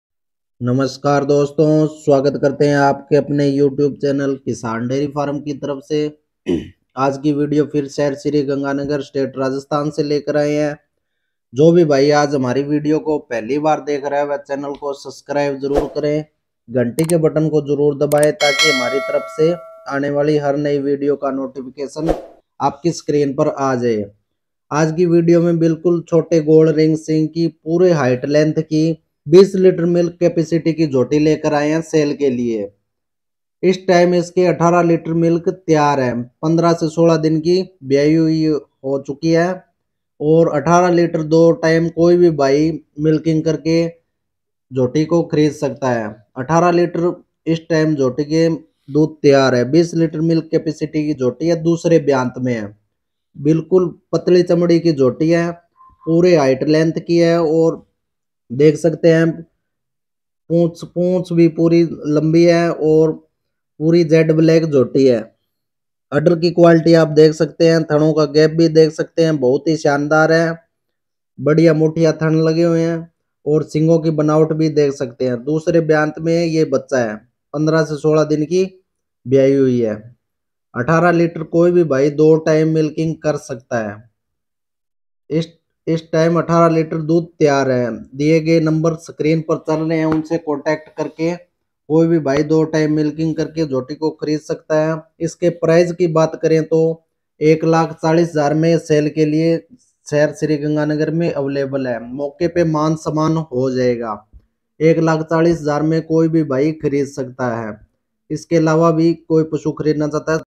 20 लीटर दूध वाली एक अच्छी झोटी बिकाऊ 2 ब्यात में शहर श्री गंगानगर राजस्थान।
चौधरी लालचंद जाट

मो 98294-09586

पता  श्री गंगानगर राजस्थान
सरकारी अस्पताल के सामने वृद्ध आश्रम रोड
गली नंबर 7
मंकान38
सिद्धार्थ कॉलोनी
सूरतगढ़ रोड डेरी फार्म
श्री गंगानगर राजस्थान